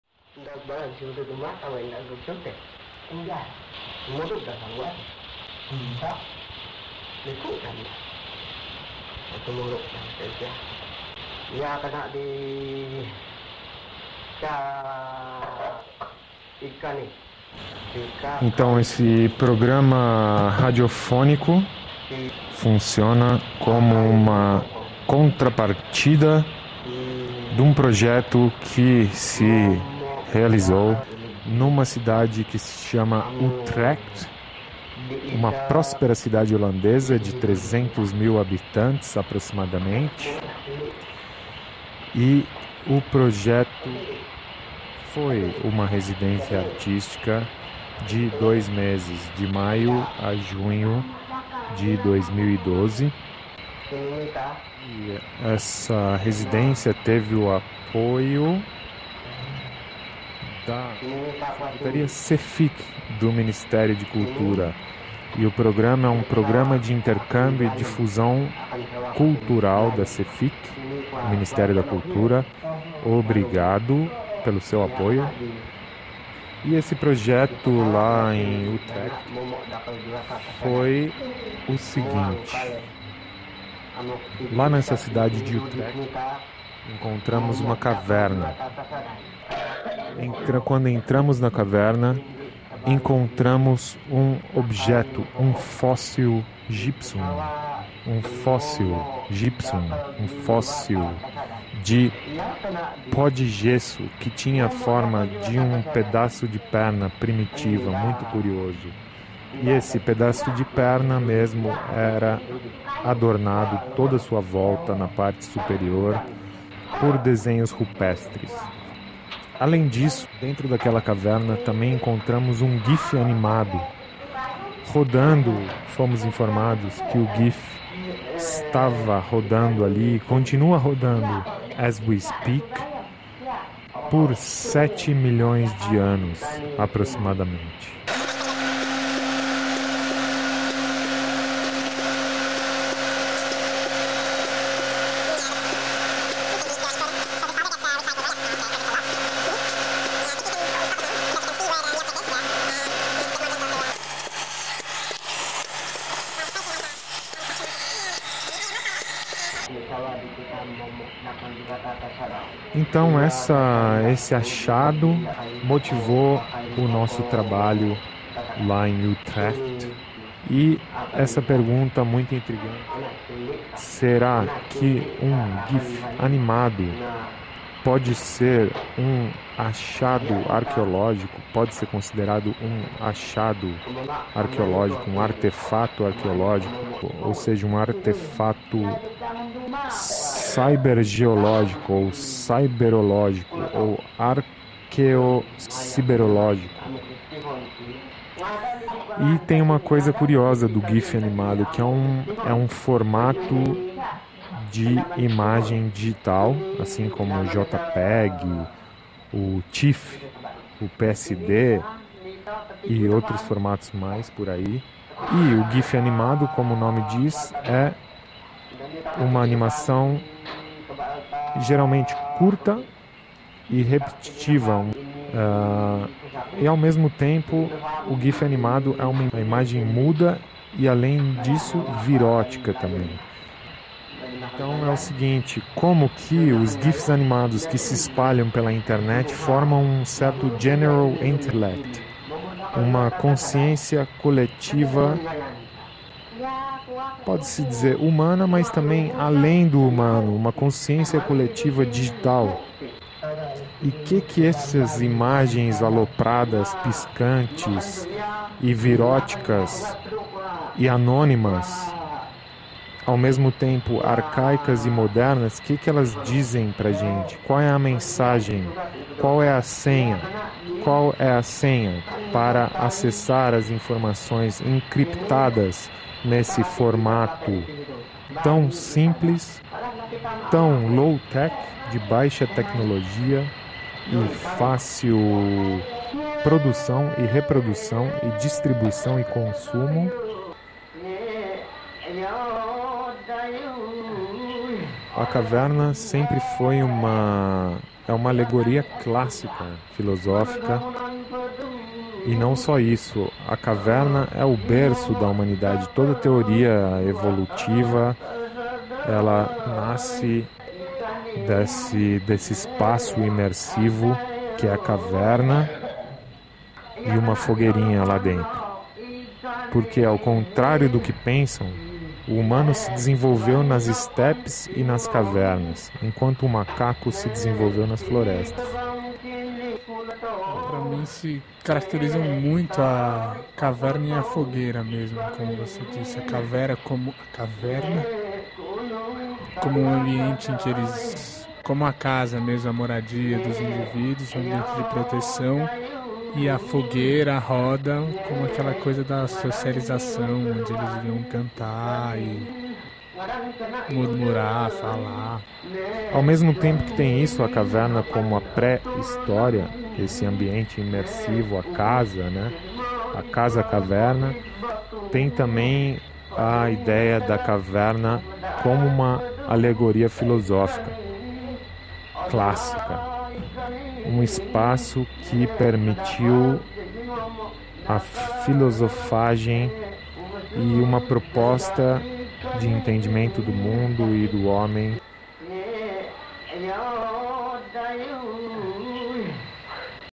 GRUNA
relato de instalação e oficina realizados por leandro cardoso nerefuh e caetano carvalho, em 2012, utrecht, holanda